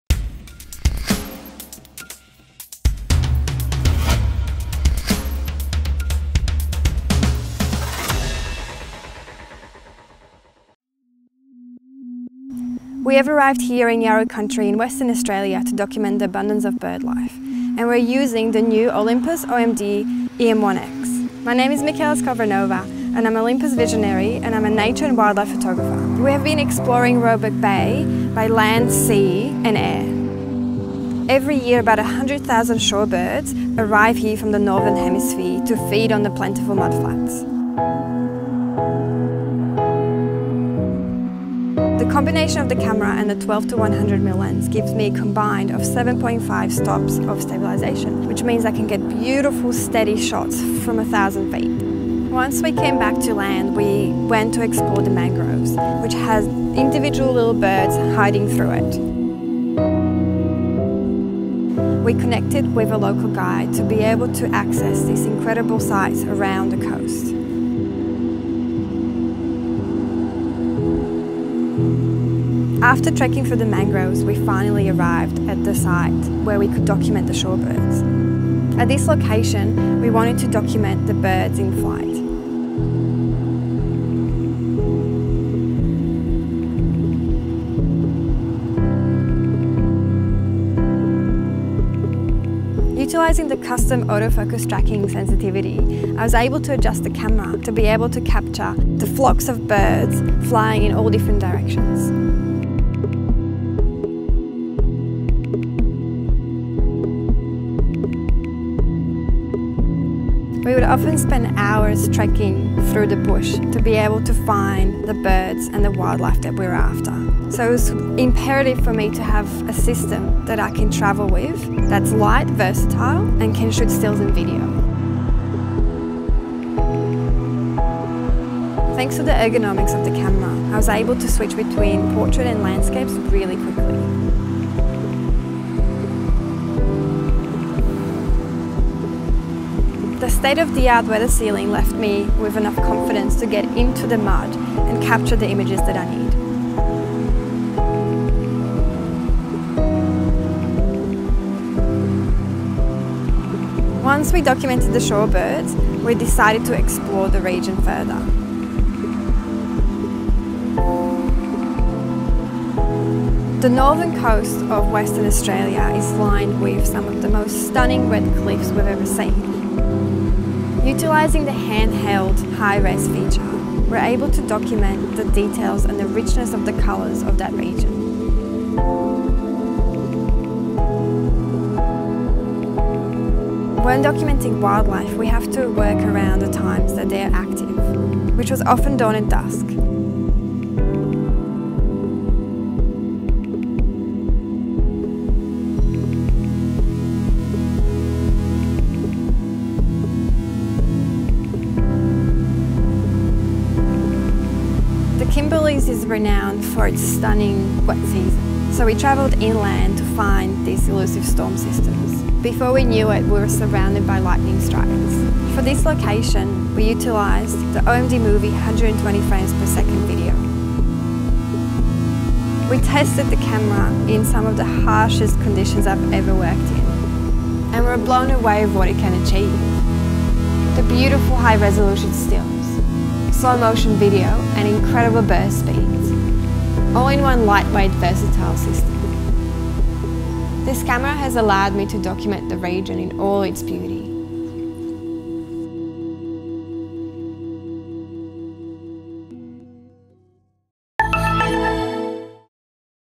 OM-D E-M1X | Nature photography with Michaela Skovranova
Explore the nature and wildlife of Western Australia together with Olympus Visionary Michaela Skovranova. Join Michaela on her expedition to capture the shorebirds on Roebuck Bay and get to know the advantages of the OM-D E-M1X.